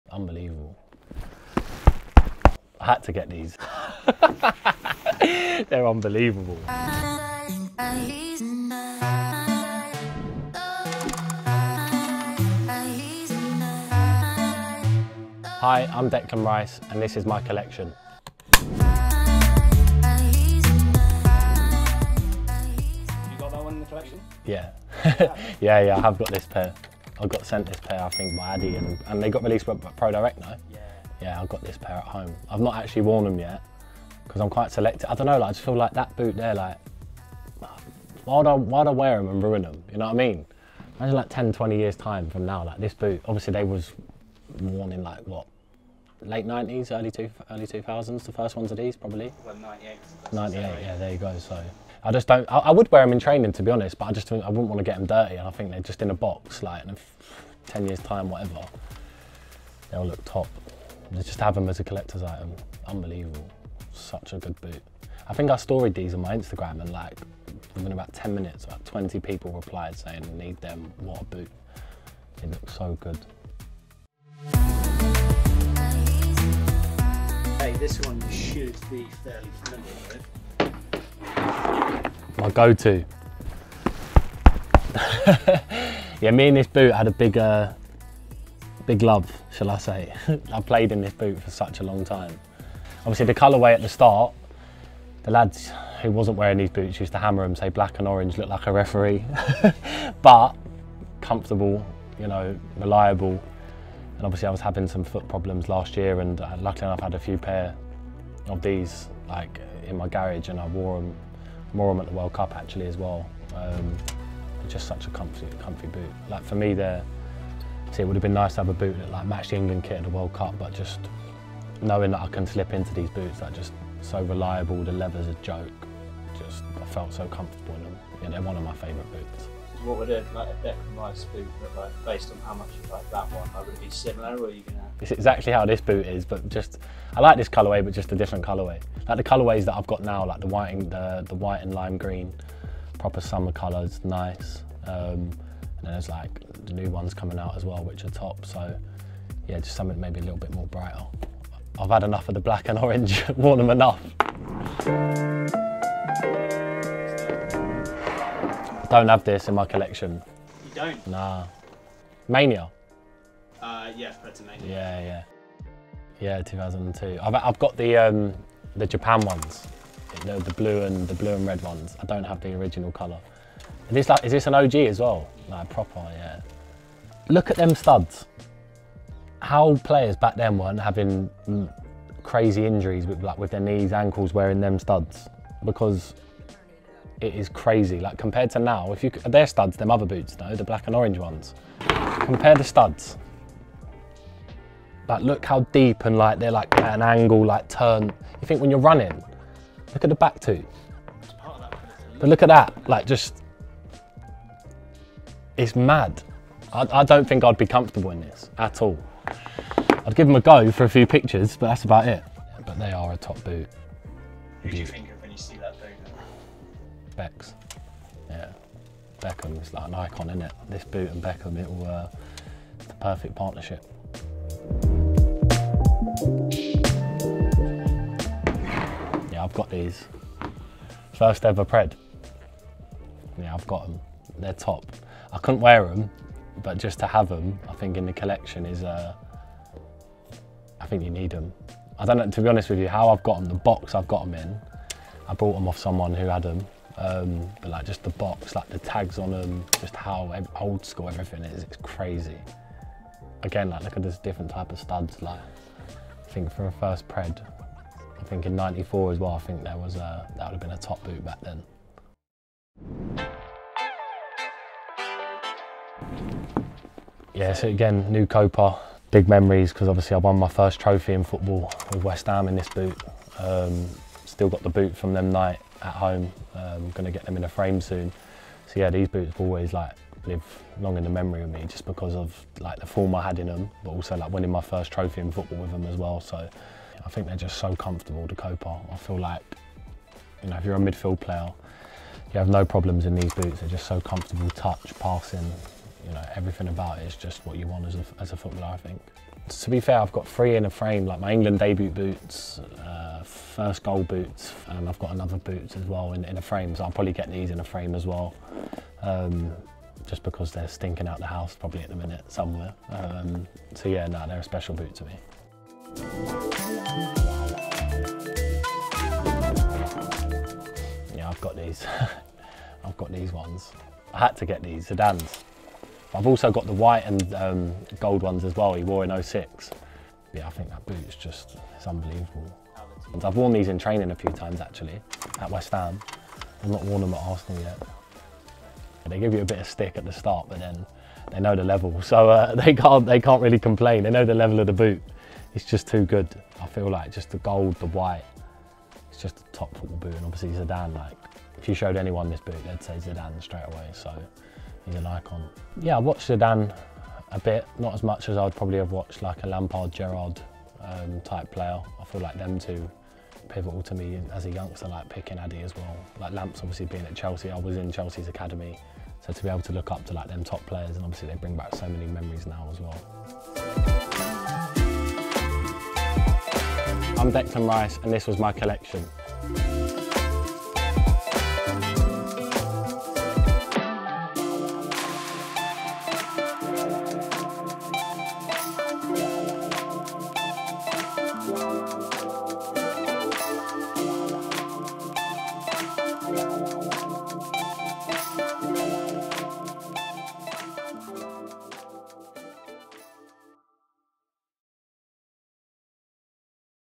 DECLAN RICE goes through his FAVOURITE FOOTBALL BOOTS ⭐🔥 | COLLECTIONS
Arsenal and England midfielder, Declan Rice goes through the boots which made him the player he is today and talks about his memories of wearing each pair. What is your favourite pair? 👇

0:00 Declan Rice's football boot collection
0:22 Declan can't wear these boots
1:17 Declan Rice's favourite boot
2:37 The boot Declan doesn't have in his collection
3:48 My first ever Predator
4:24 Winning first Trophy
5:20 Wearing Gold Boots?